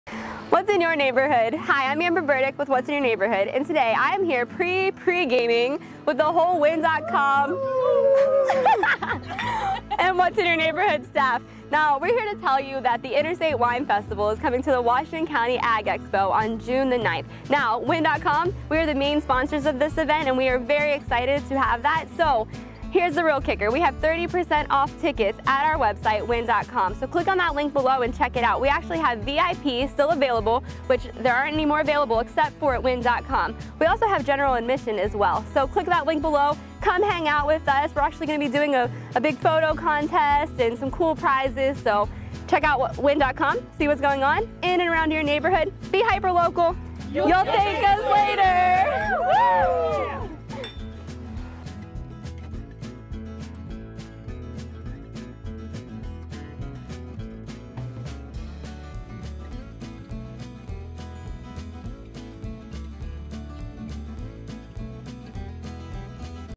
Wine Fest Ad
Save The Date! June 9, 2018

You receive free parking, entry into the festival, live music all day, a “wine drinker” wristband (which allows you to sample from all the wineries), and a special festival wine glass.